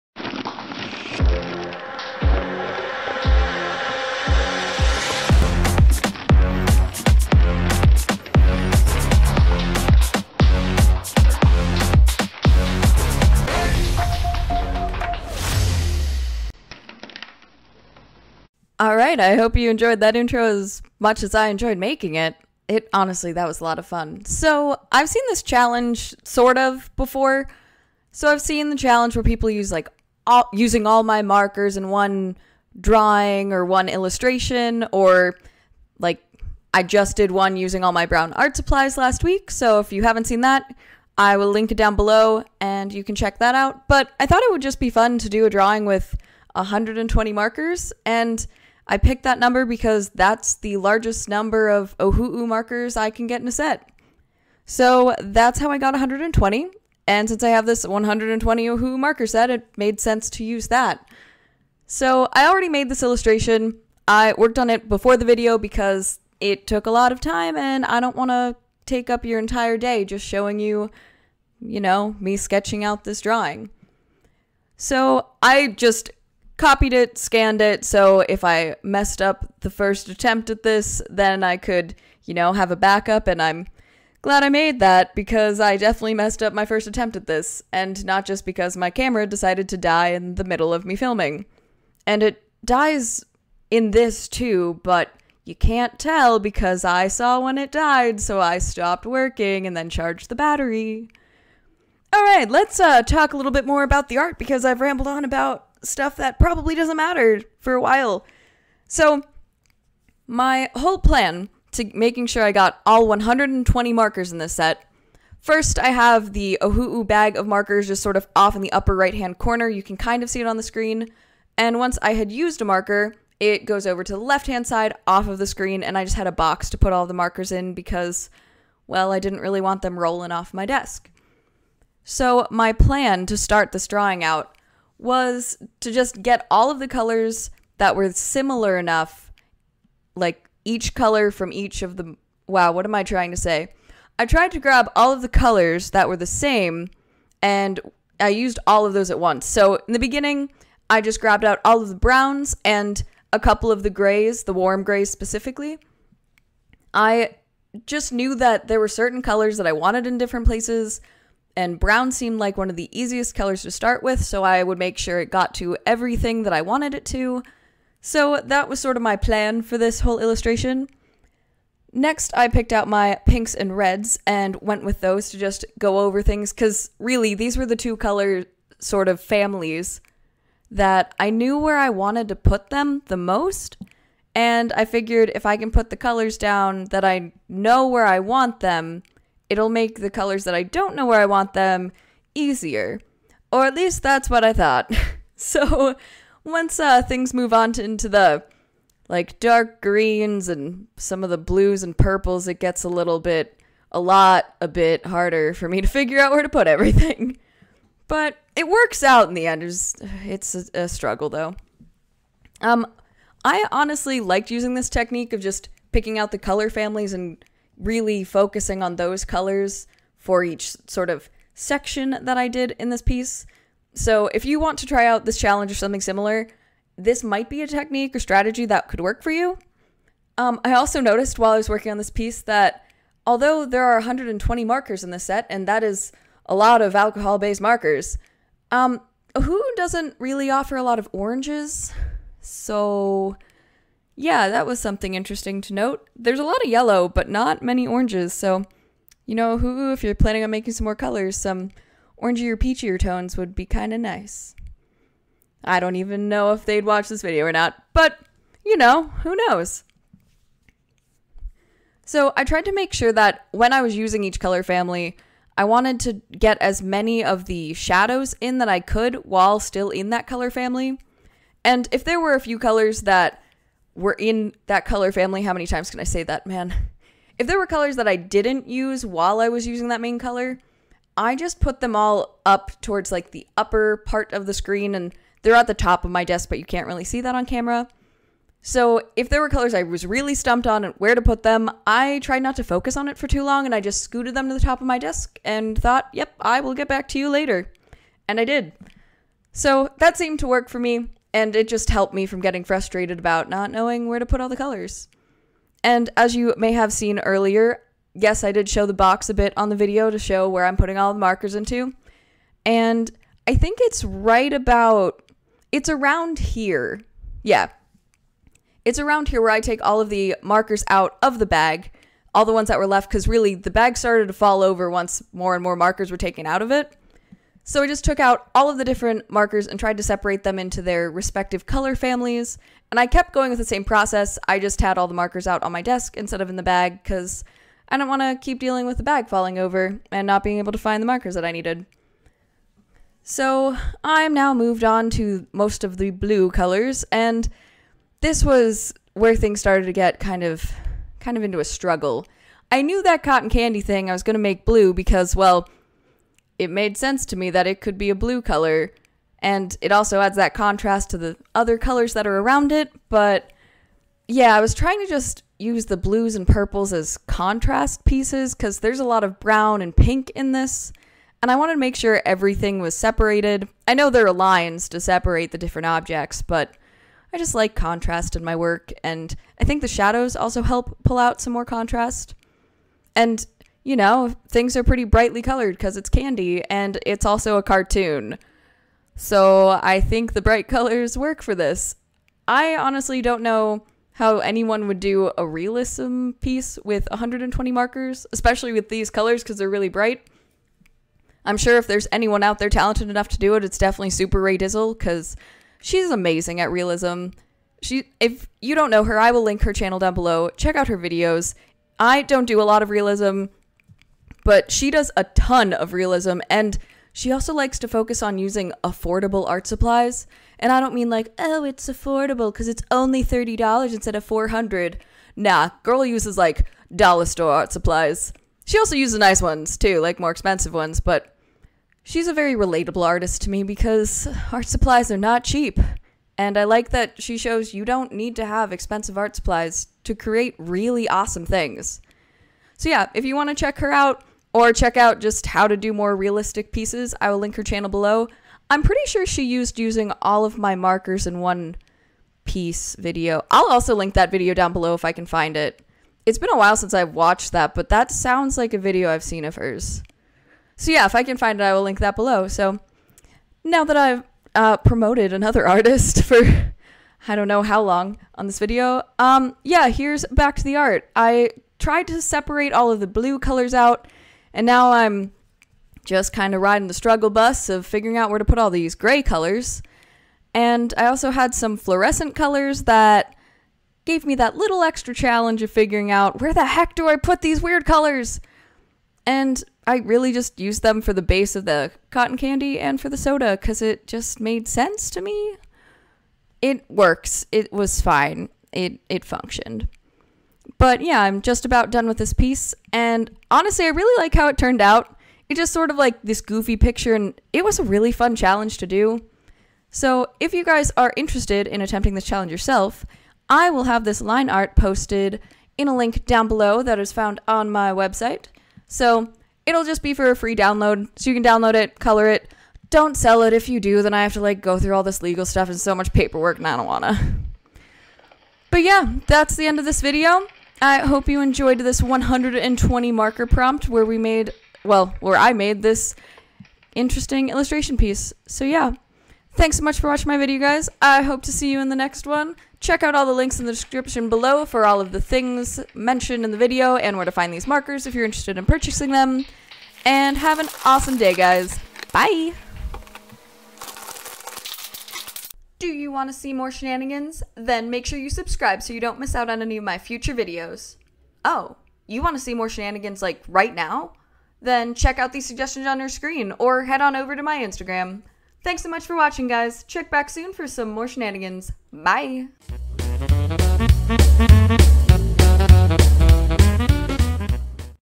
Drawing with EVERY SINGLE Ohuhu Marker + FREE Coloring Page
Hi guys! Shananigans here!

In this video I create an illustration using all 120 of my Ohuhu Chisel Markers!

I had a lot of fun creating this piece! This video is not sponsored, but hey, I won't stop a company from supporting my work if they so choose. *wink wink* *nudge nudge*

MATERIALS USED:

Neenah Exact Index Premium Cardstock:

Ohuhu 120 Marker Set:

Get your own copy of this coloring page to print and color here:

~ Get a Copy of My Coloring Book ~